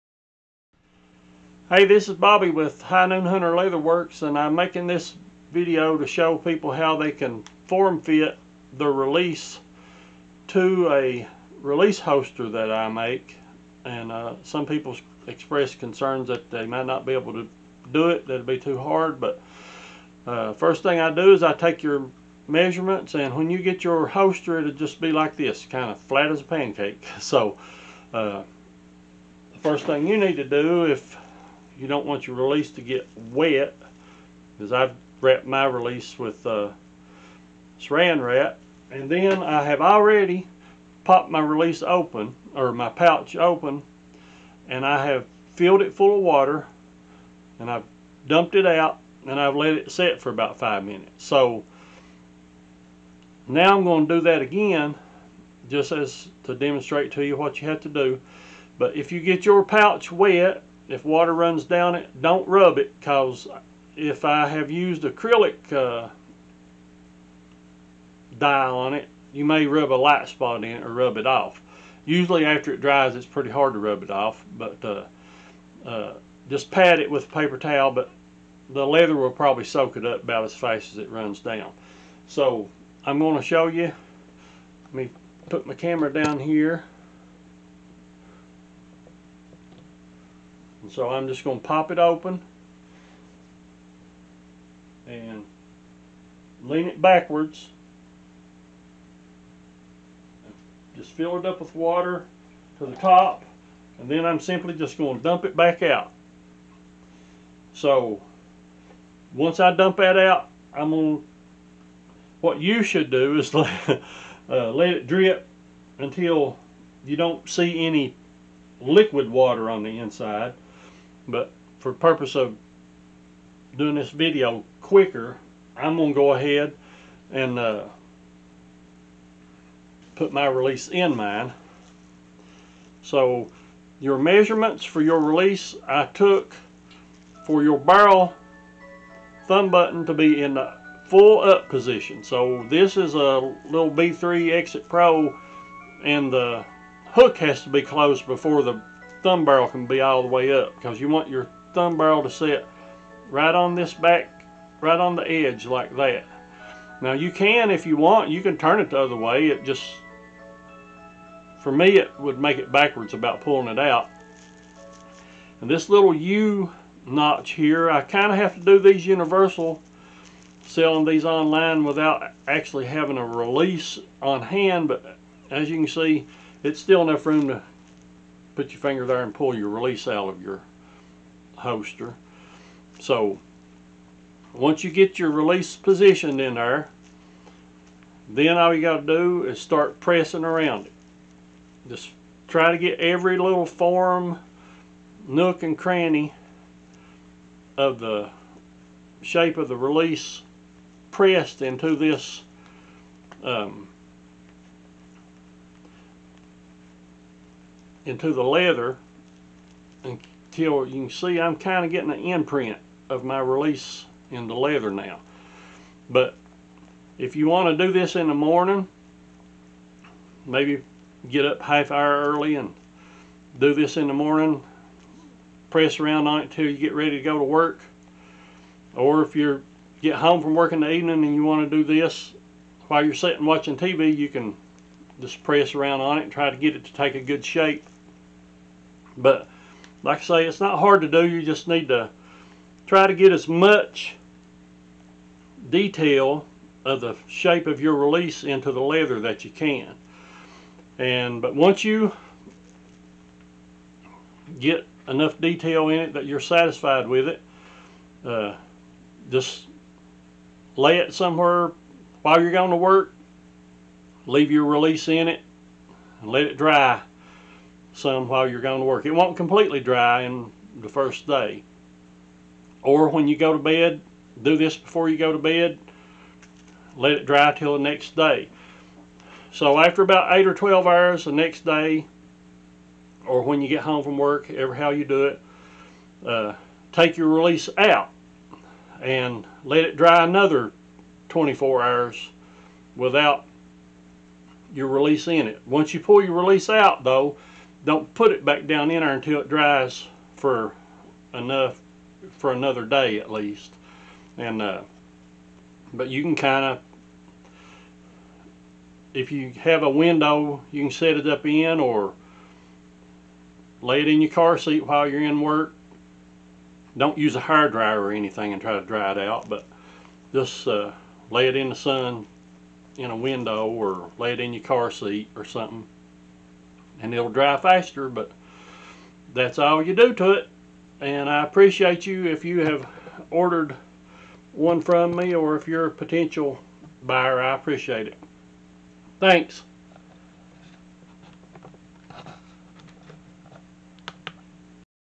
Forming a release holster
How to form your custom Highnoonhunter Leatherworks Release Holster.